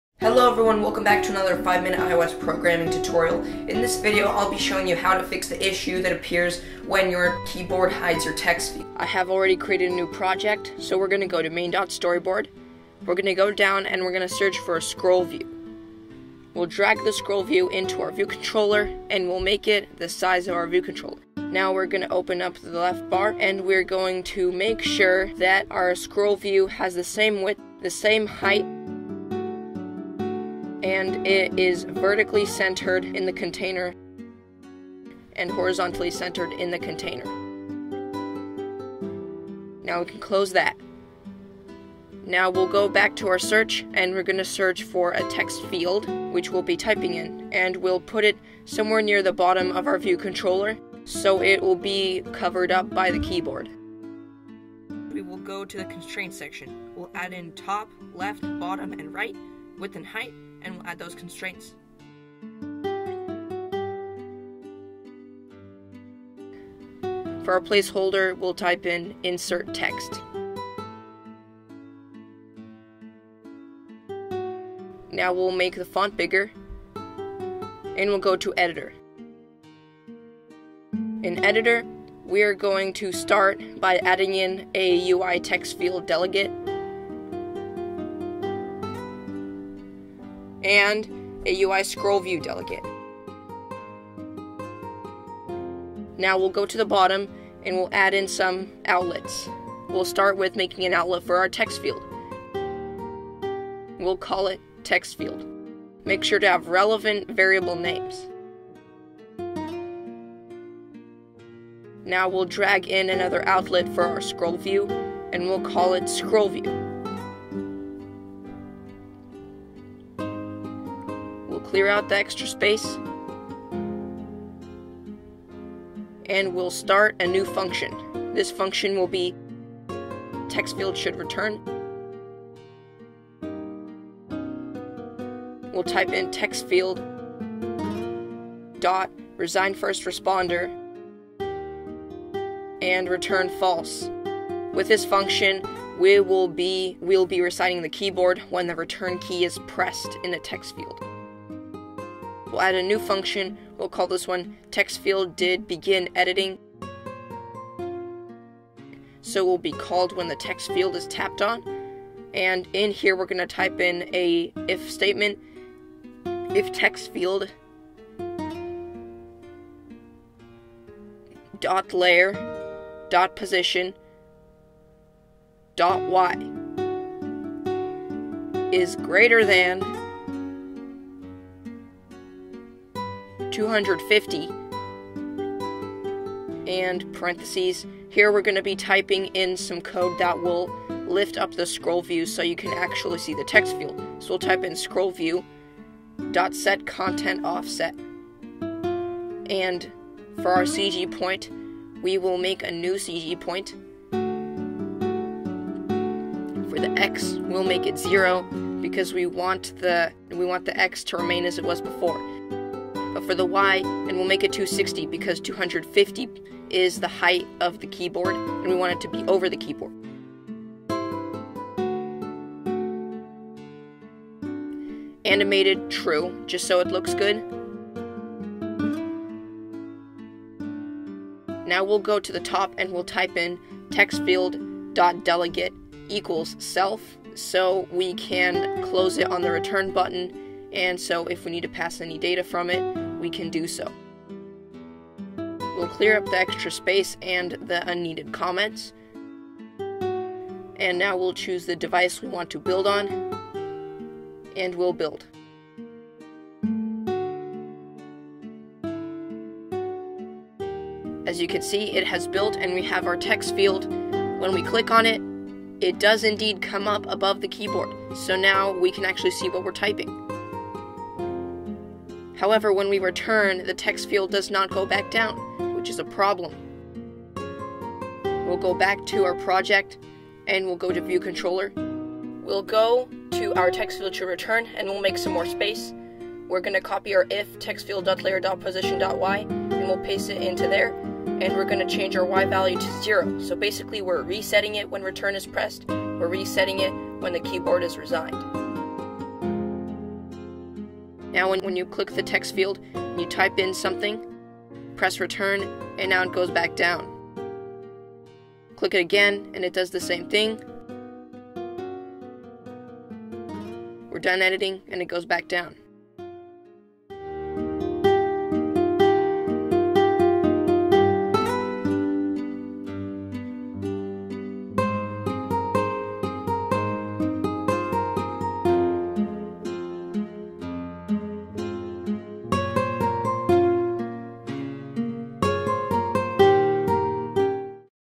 Xcode iOS Swift Programming Tutorial - How to Fix the Keyboard Overlapping the Text Field Issue
This is a big problem that many iOS developers will most likely encounter. Luckily, this problem can be fixed with some simple code.